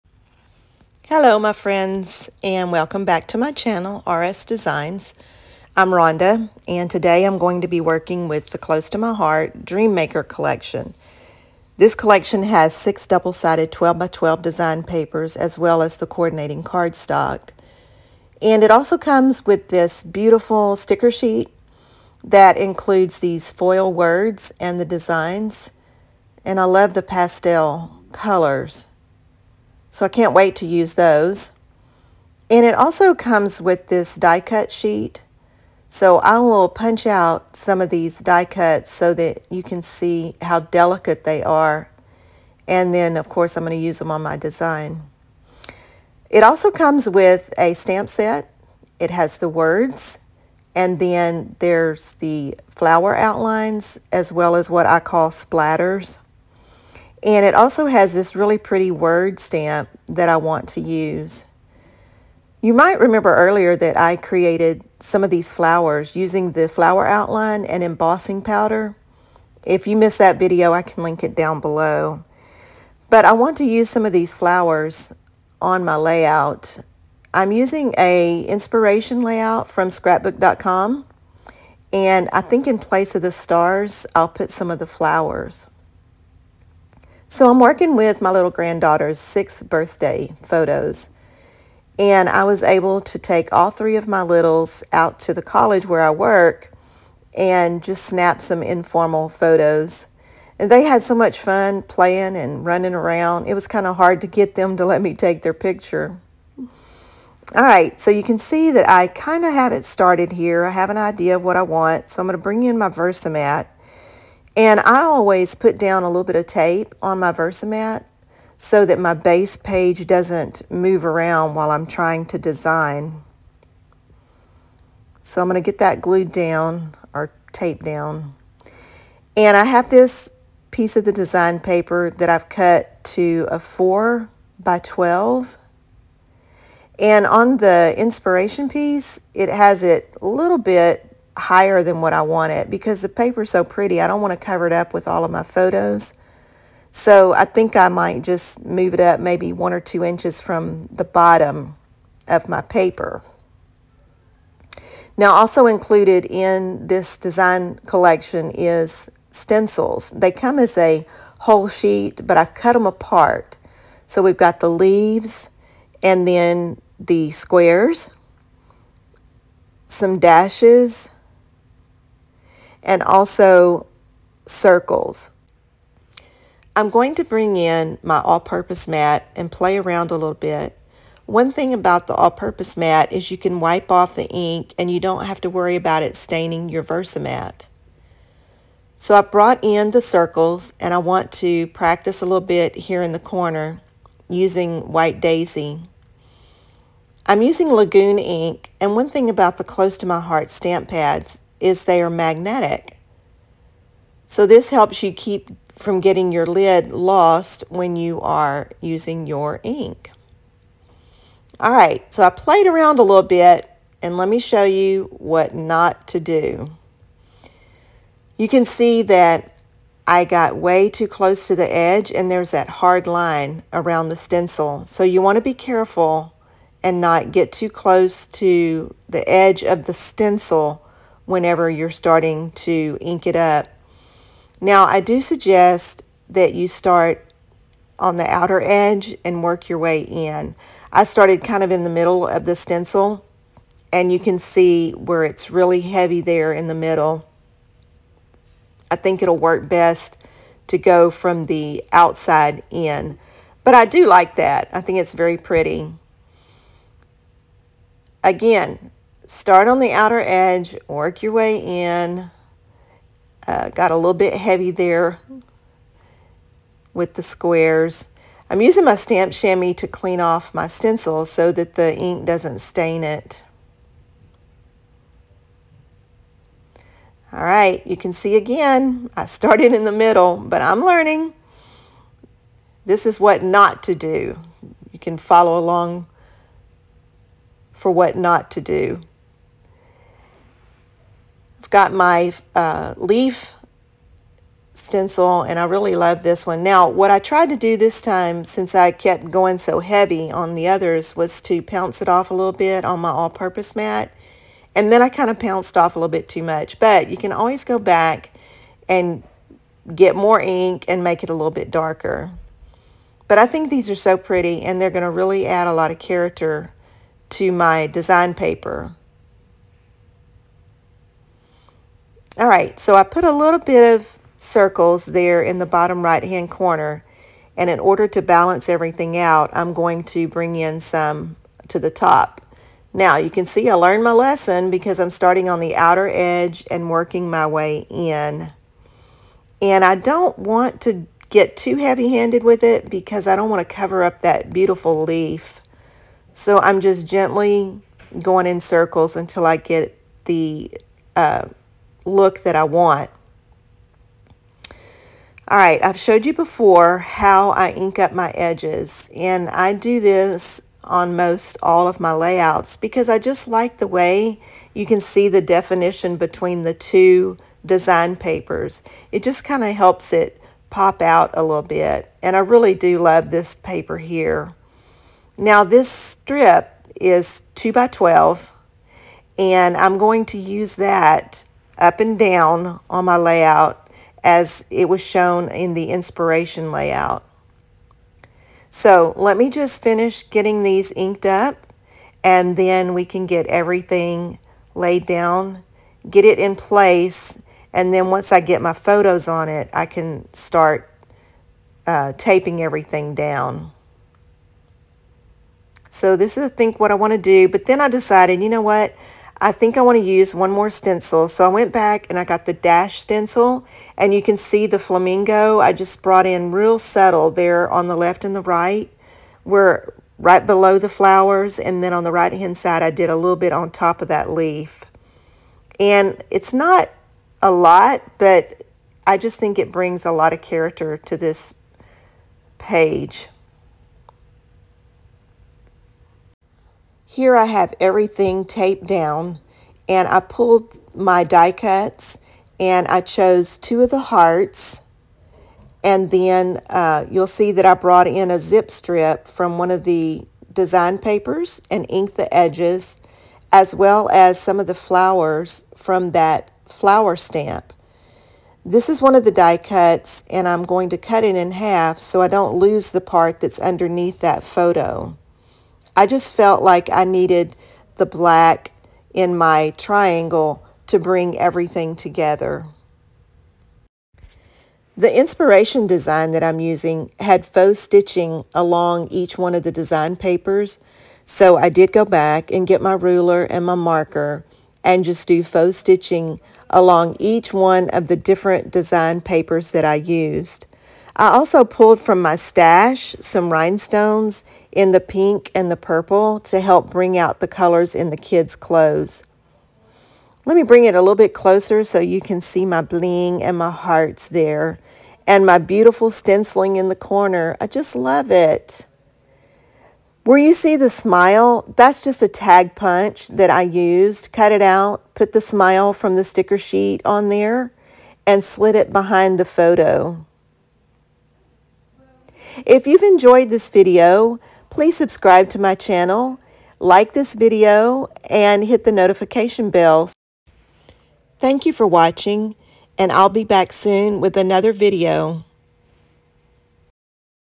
Dreamy DreamMaker Layout
A layout created using Close to My Heart Dream Maker collection.